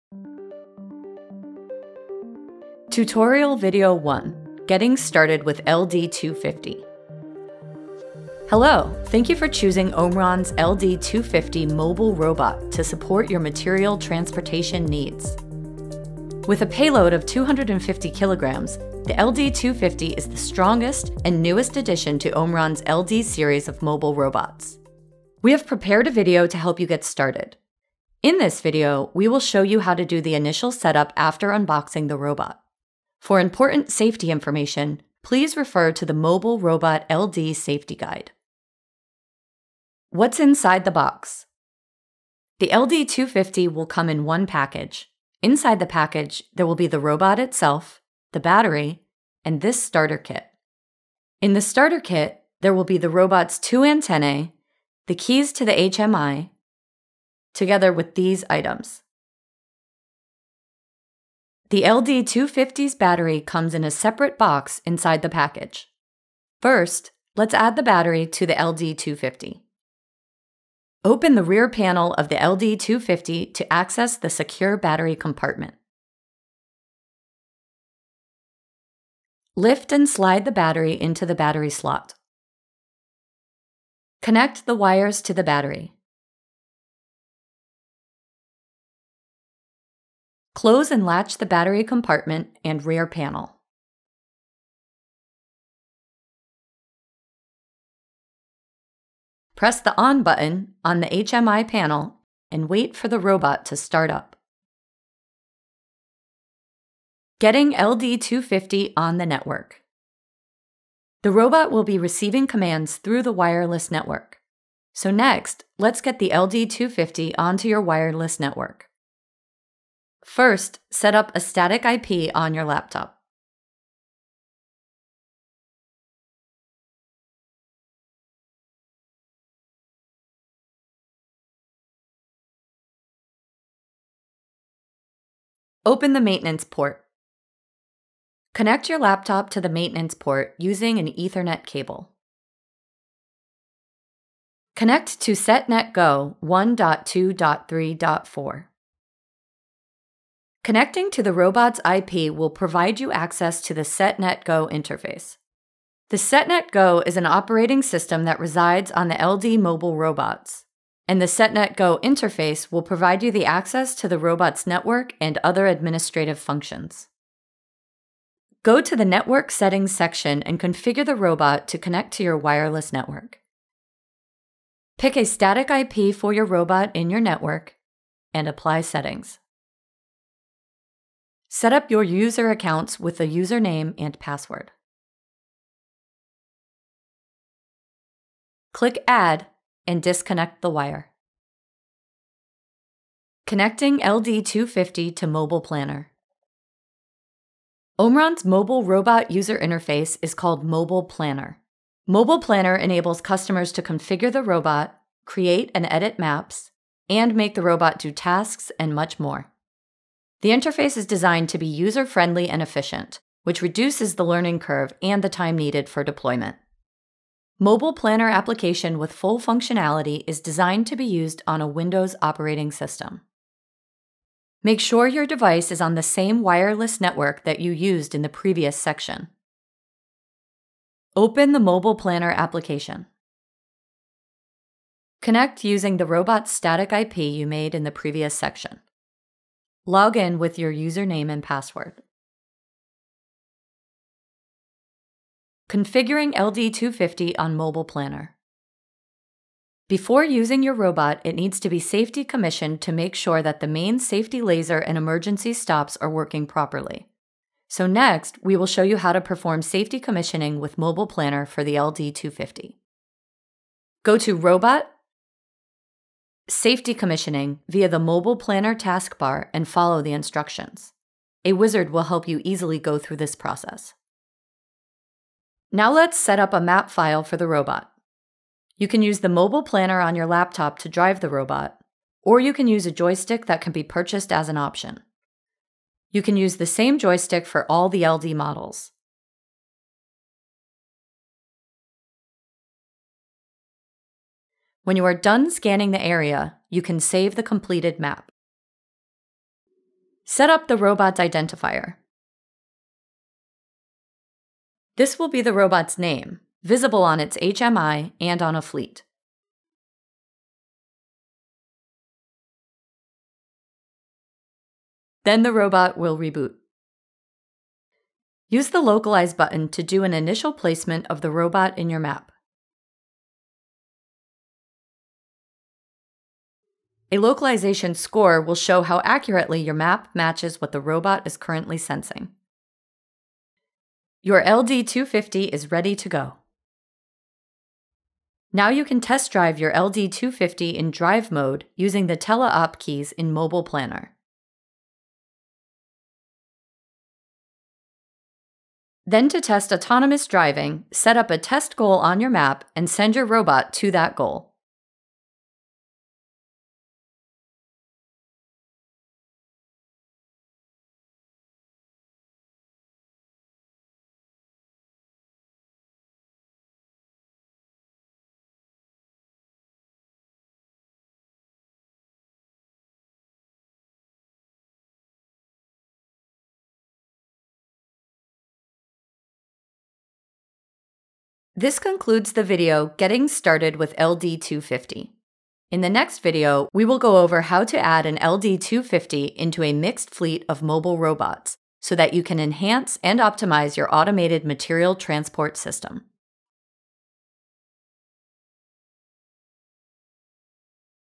LD-250 mobile robot tutorial 1- Getting started
In this video, you can learn how to get started with the LD-250, from unboxing the mobile robot to getting it on your wireless network, and connecting to the MobilePlanner. Get your LD-250 ready to go!